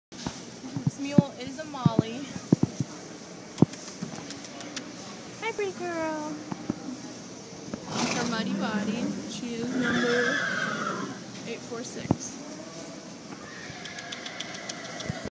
7-23-8-846- Chestnut Mule molly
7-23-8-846- Chestnut Mule molly recommendation was she was a plow mule/buggy mule. Approx 15 hands, smooth mouth.

$485.00